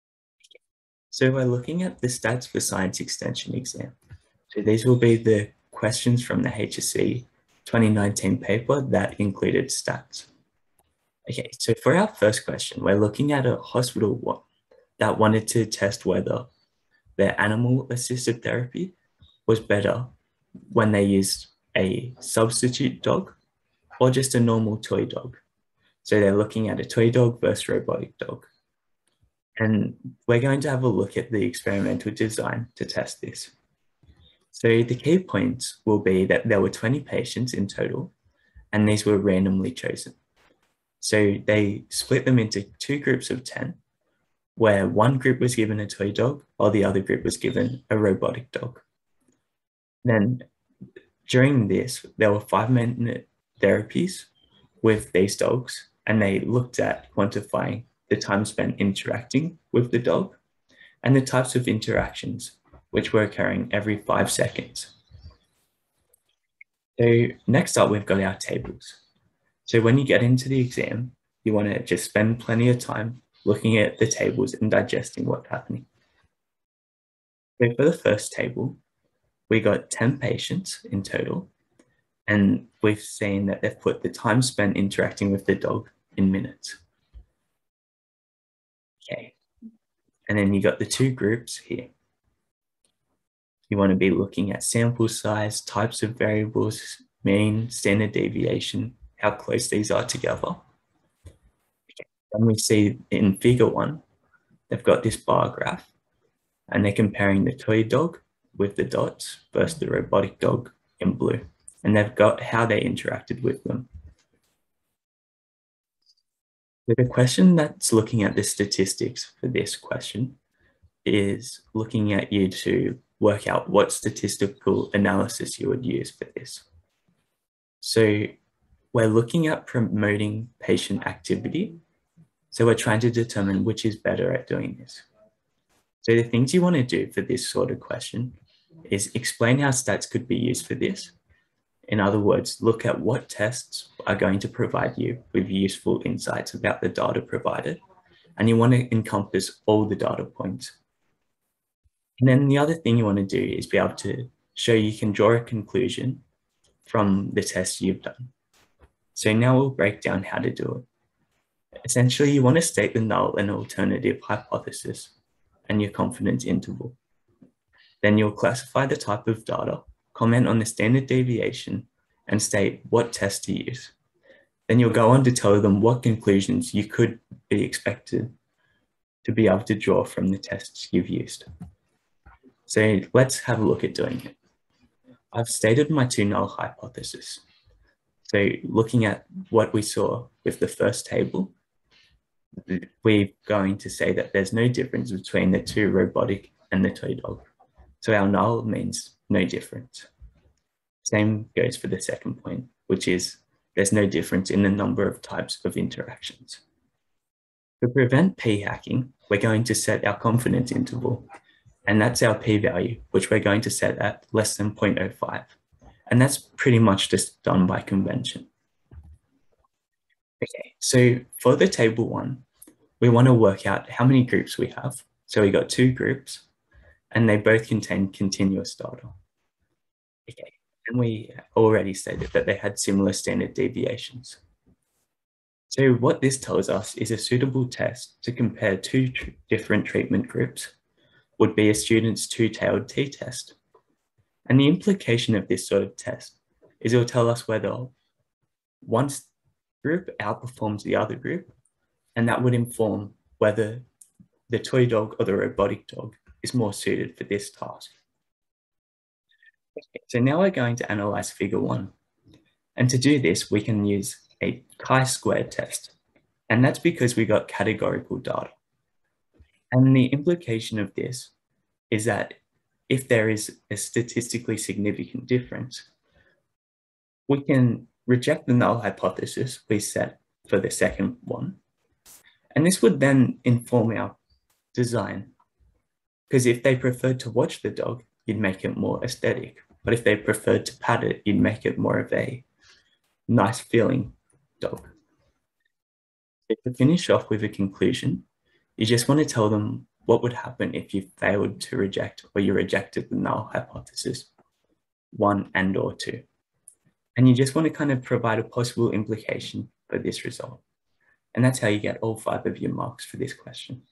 2019 HSC Science Extension Exam Answers Statistics Question
Video detailing how to do question 2a of the 2019 HSC Science Extension exam.